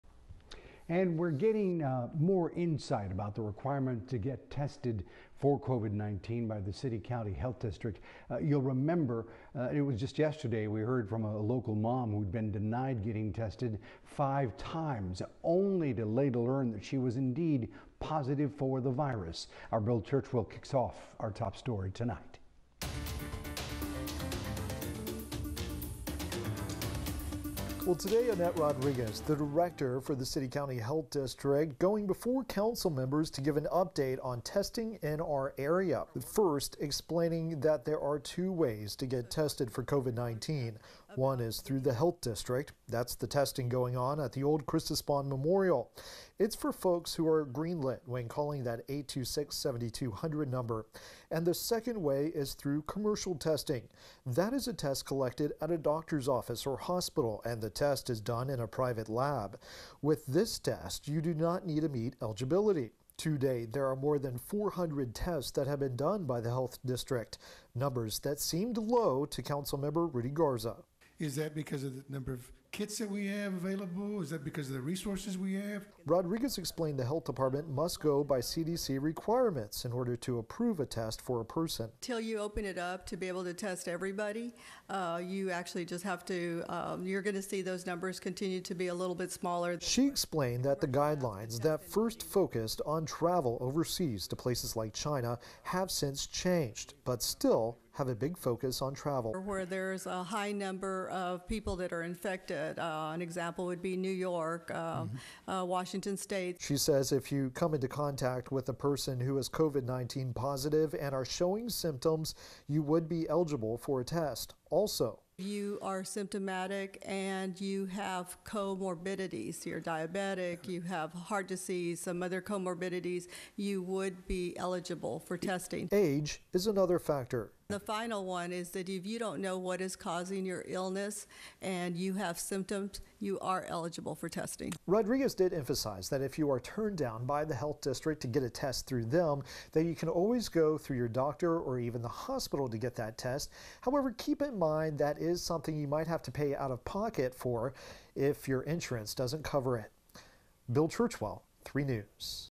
Insight into the requirements to get tested for COVID-19 by the city-county health district
Just yesterday we heard from a local mom who had been denied getting tested 5 times only to later learn she was indeed positive for the virus.